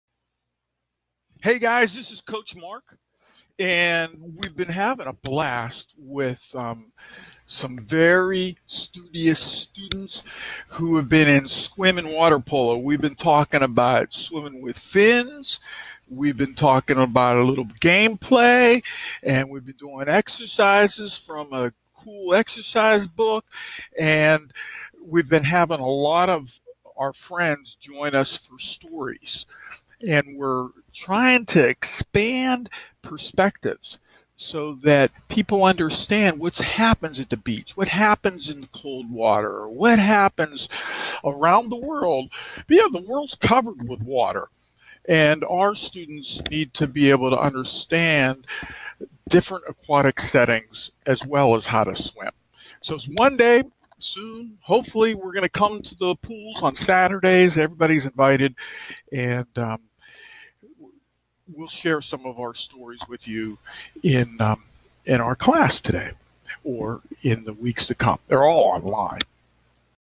Promo for SKWIM & Water Polo final day
SKWIM & Water Polo in Summer Dreamers in 2020 went full on as a Cyber Swim Camp. Students were great as we shared stories of water and discussed the lessons and valued take-away words, expressions, and skills.